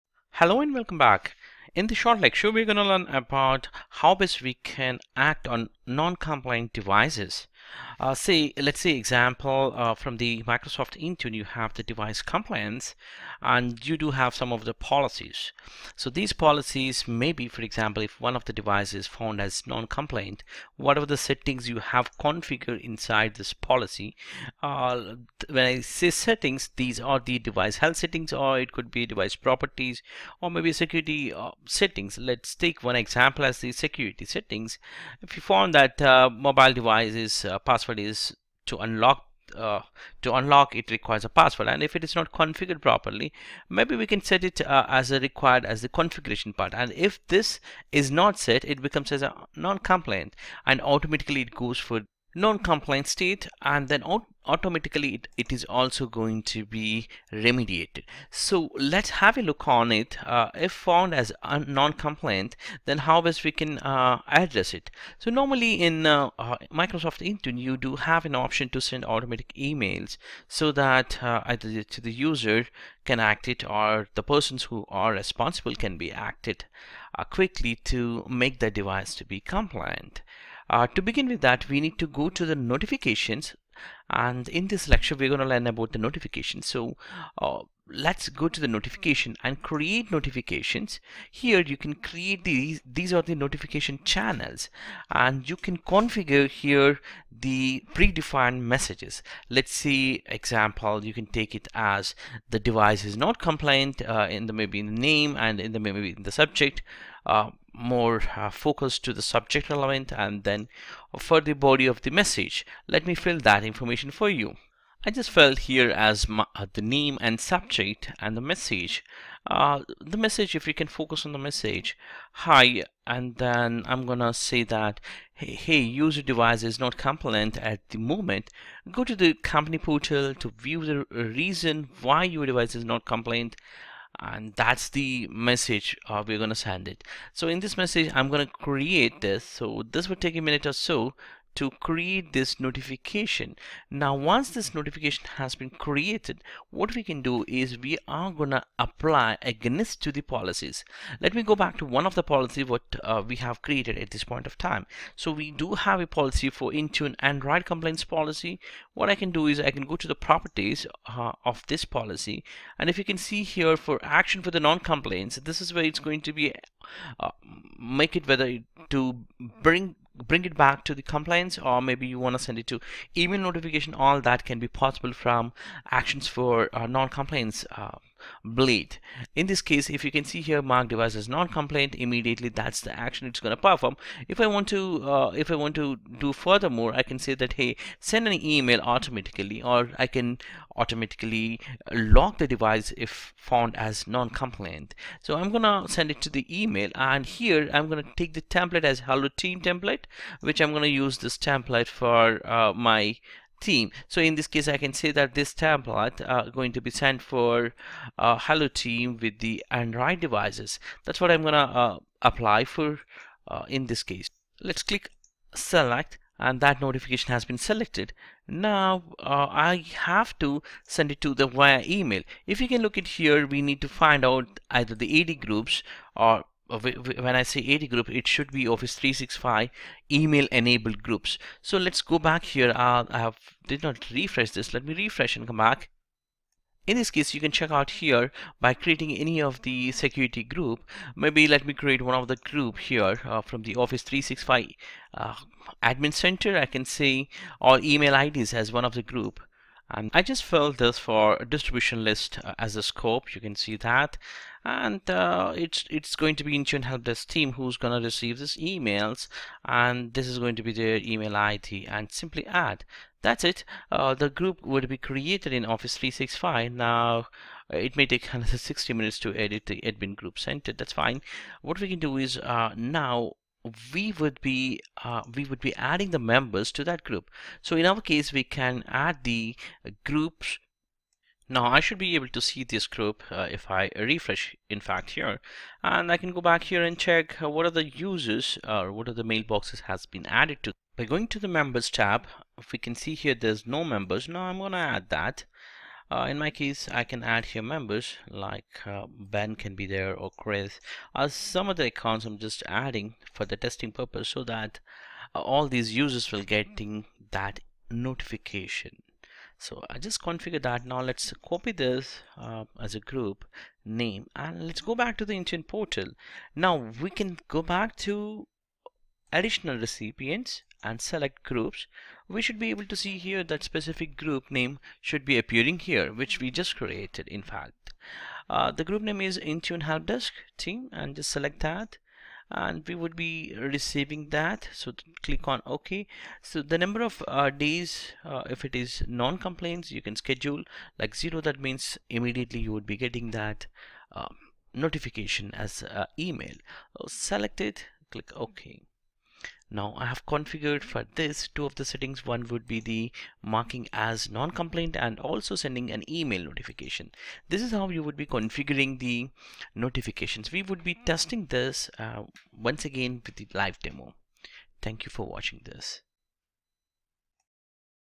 Device compliance - Notifications - Microsoft Intune Training Series video No#31
Microsoft Intune Training Series video No#31
by PaddyMaddy